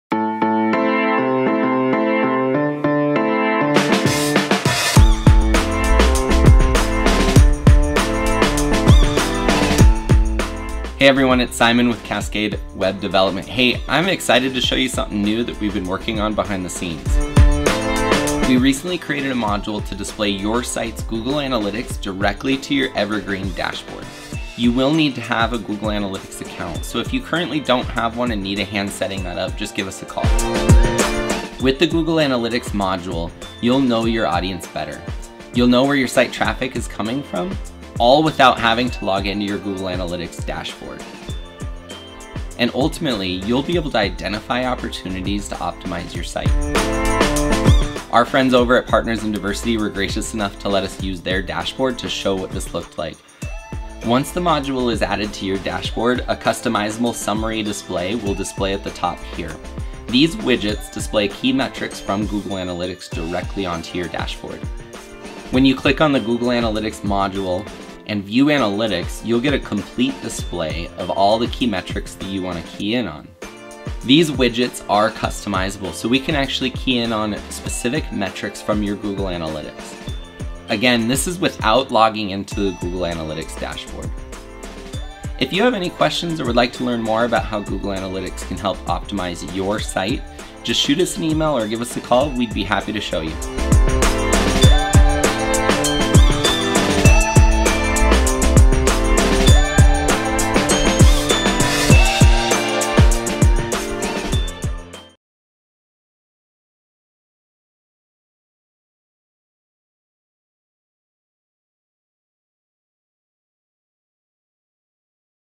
Google Analytics Module by Cascade Web Dev
Introducing the Google Analytics Module that gives Evergreen users a customized dashboard using widgets to display key metrics.

- Know your audience better
- Know where your site traffic is coming from
- Identify opportunities for optimization of your site
- No need to log into Google

If you have any questions or would like to learn more about how the google Analytics Module can help you and your site, give us a call or email us at